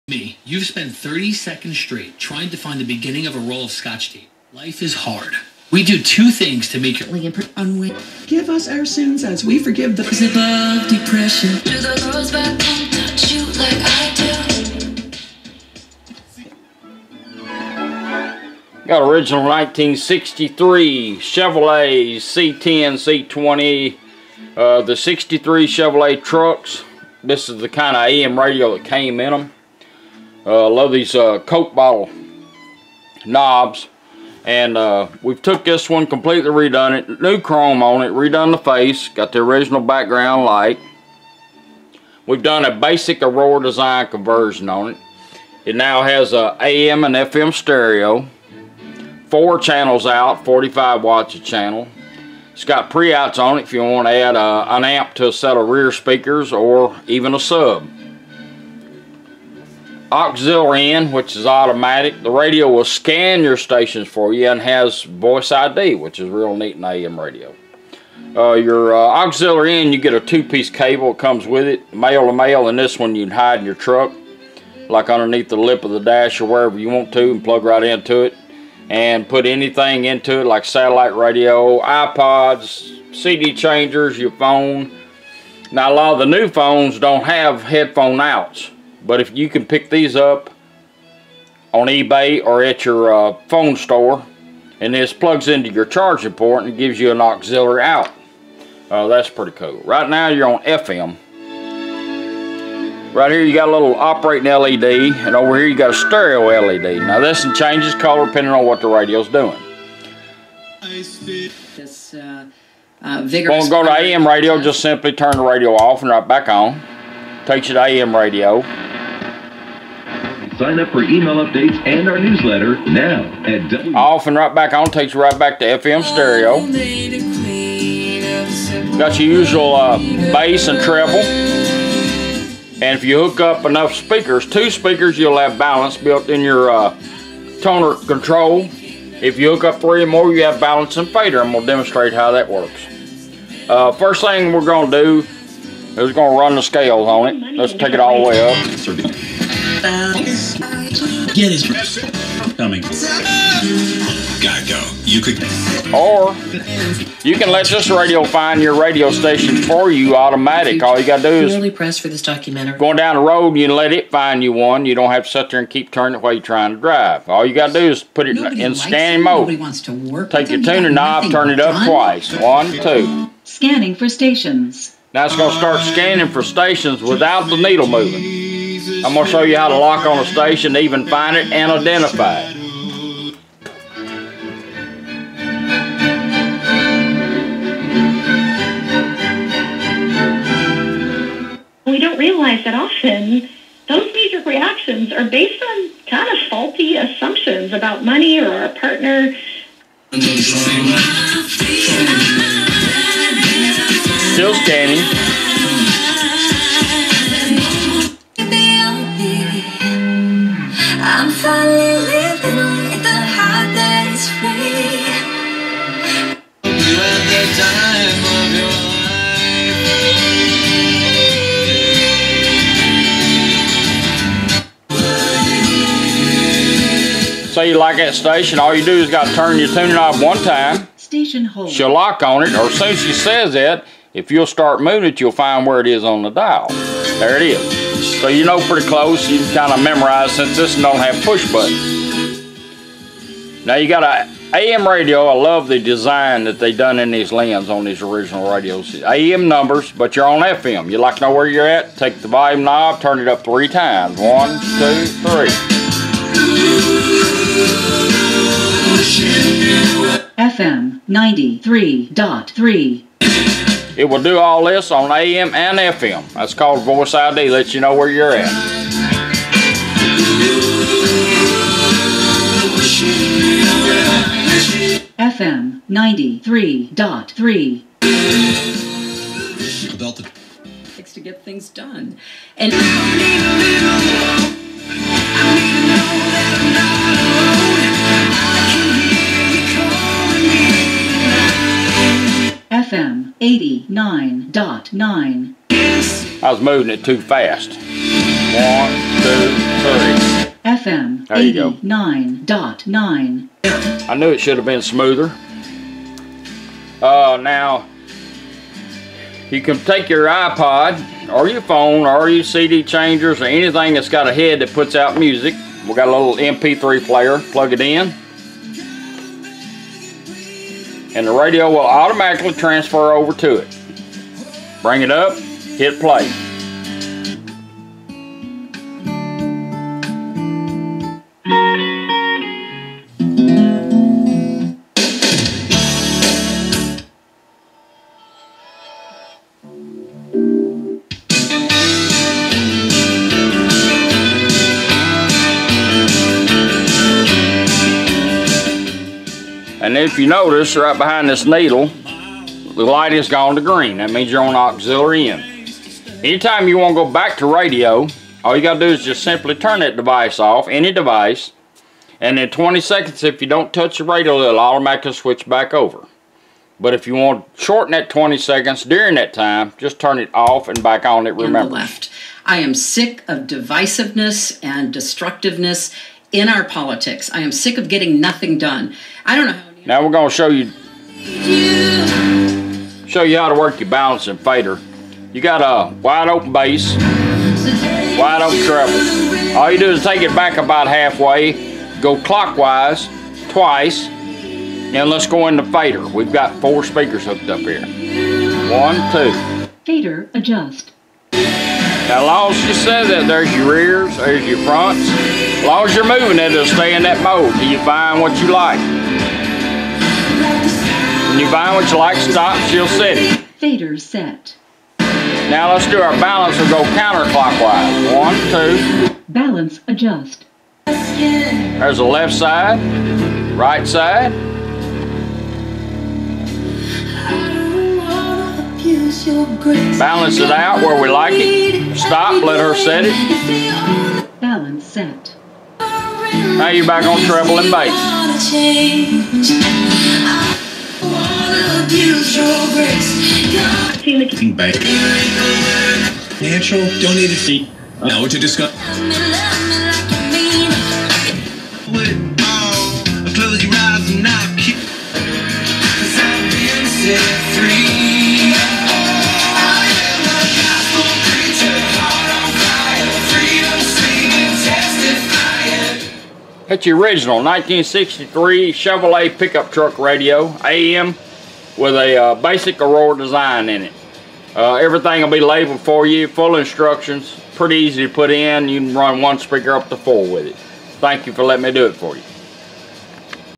1963 Chevy C10 truck AM radio
Custom built by R & B Vintage Auto Radio Powered by Aurora Design products now has AM/FM stereo/Aux input /Scan/Voice ID four channels 45 watts per channel, RCAs pre line outputs for adding amps and a Sub all in the original 1963 Chevy C10 AM radio. Get your classic radio updated today.